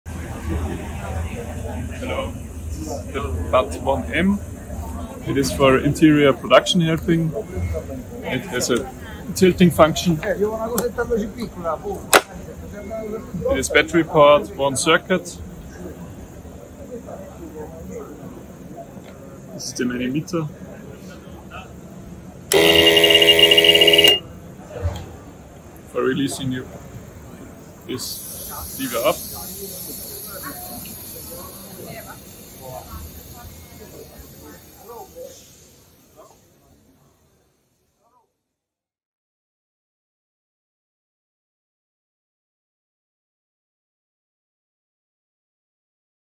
Barbaric Battery Lifter BAT 1M //
Stefan Michael Barbaric is the technical engineer who developed and designed many of our mobile battery lifters.
In this video, he presents our small and compact battery lifter BAT 1M on the Glasstec 2022 in Düsseldorf, Germany. It is the optimal solution for fast and effortless indoor lifting applications. It can handle various materials through its flexible sealing lip up to 125kg on a crane hook. 💪